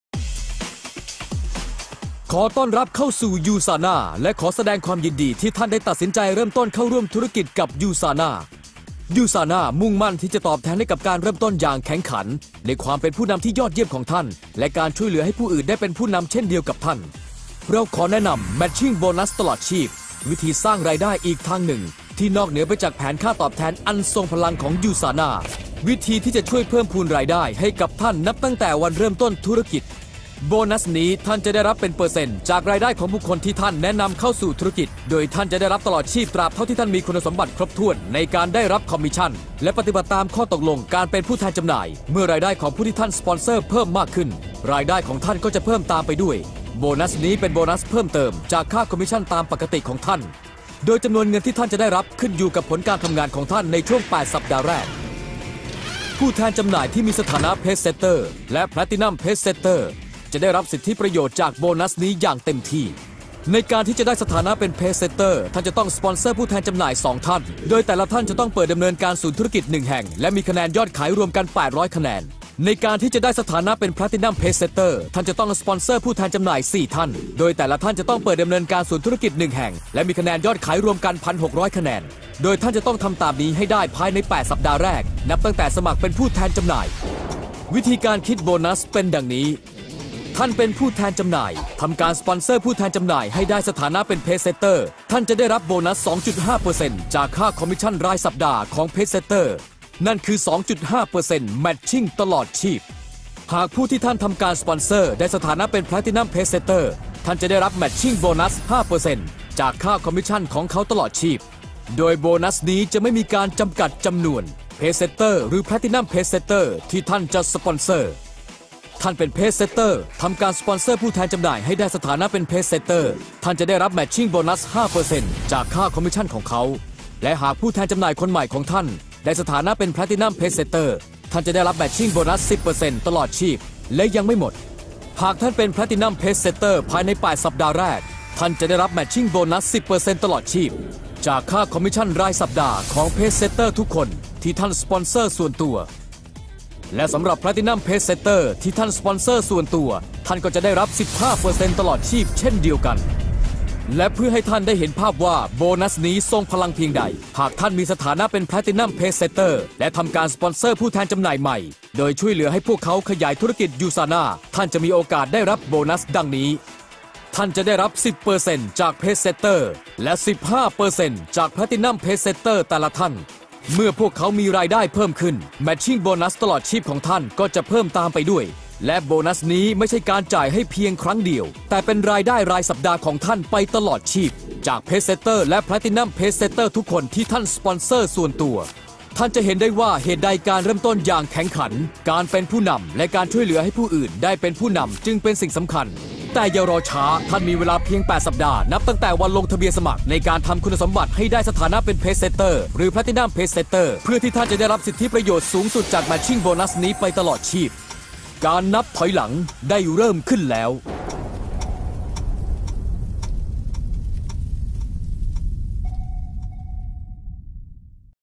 USANA Lifetime Matching Bonus 2013 (Thai Version)
One of the most important and powerful USANA incentives is the Lifetime Matching Bonus.  Check out this video to see why it can be such a lucrative part of a USANA business, especially for new Associates.

Introduction to USANA - India, Indonesia, Brunei, Singapore, Malaysia, Philippines, Thailand, Australia, South East Asia, Asia Pacific, United Kingdom, United States of America